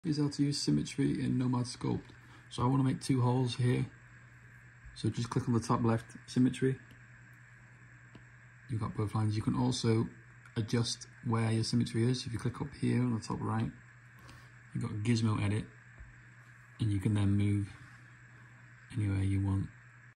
How To Symmetry In Nomad Sculpt, BEGINNER!
No mucking around quick pointer for absolute beginners in nomad! Have a great day!

Follow for more iPad artist tips @buntrockstudios